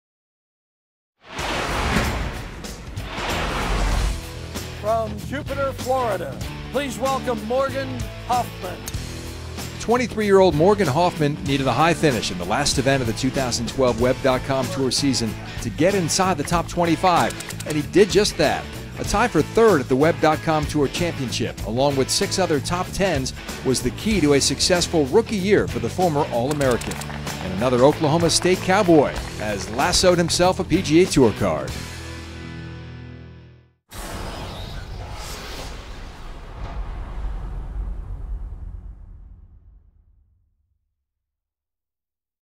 The 25 of 2012: Morgan Hoffmann
A T3 finish at the last event of the 2012 Web.com Tour season along with six other top-10 finished put Morgan Hoffmann at 19th on the money list earning him a PGA TOUR card. .